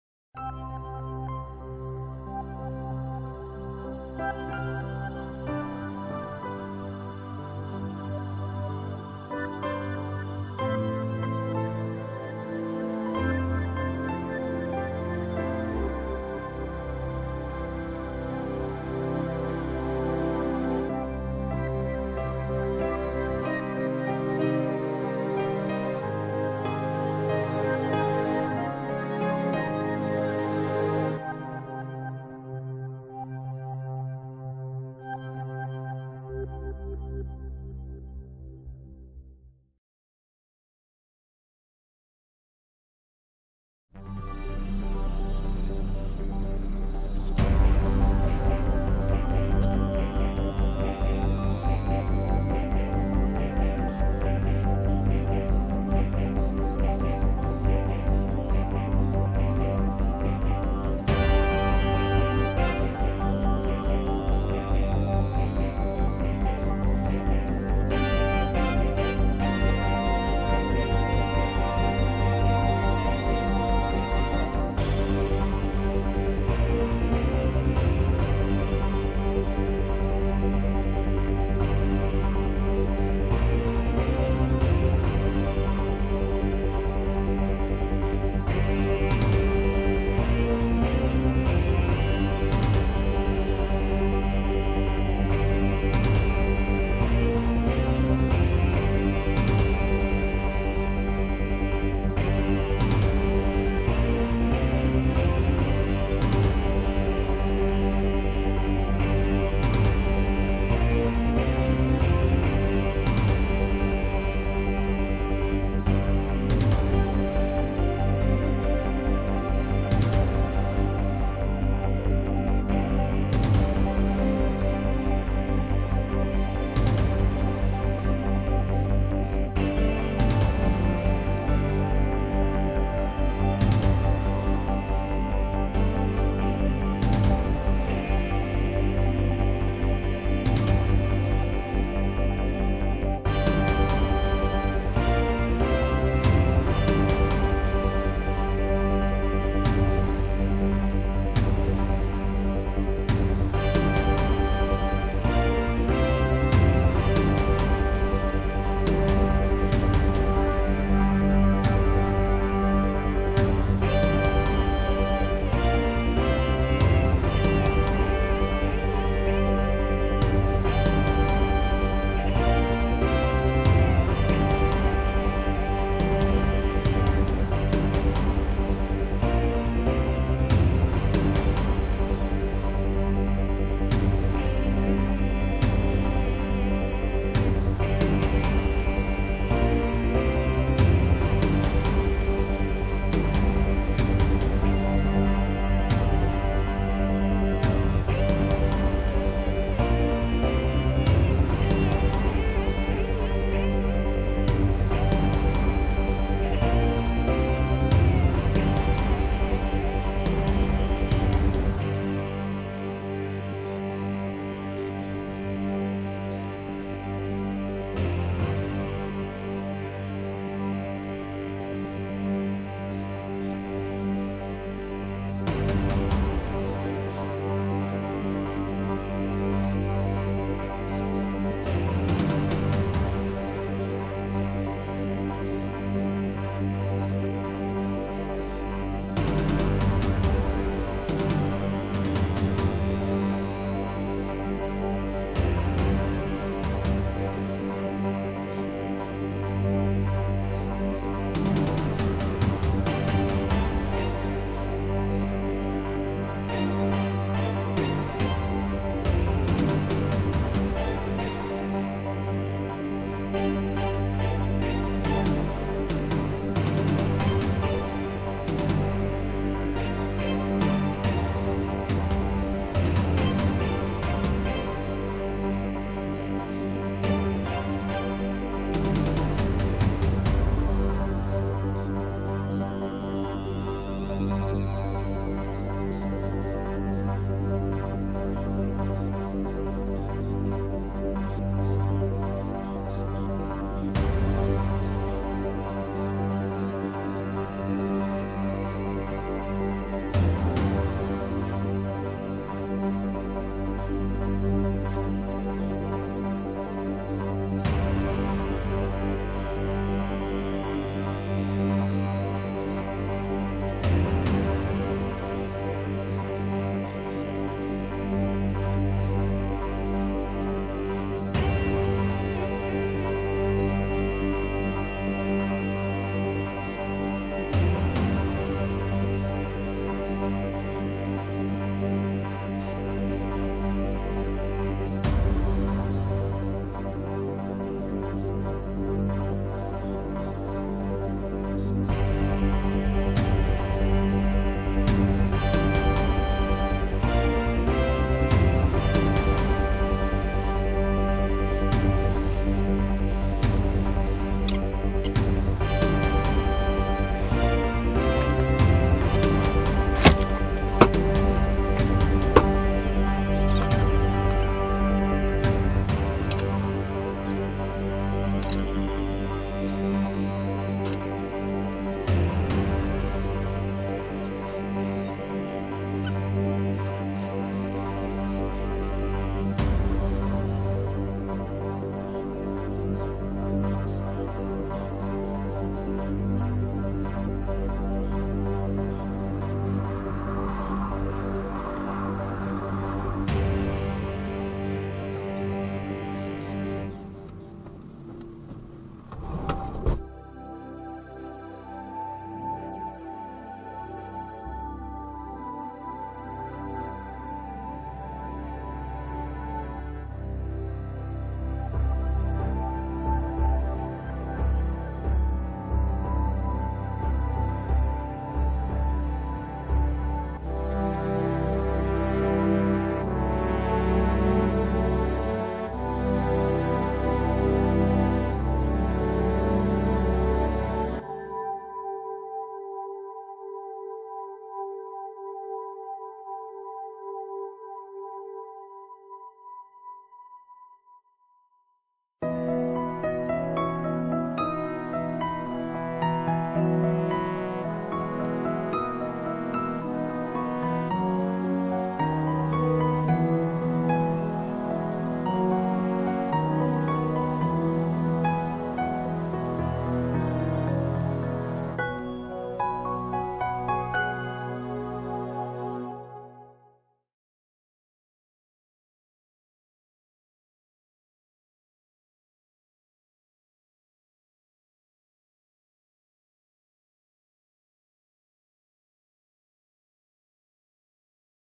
2022 DIY(Not!): Aging 2016 Ford Transit Gets a Window Tint Makeover by the Pros
November 17, 2022. The window privacy film on our Ford transit is evidently sun-damaged. The shade has turned purple over time. The ability to filter light and heat is diminished. It is scratched on many areas within. Hence, the van is due for a window tint makeover. Not my DIY project. I yield the job to the professionals.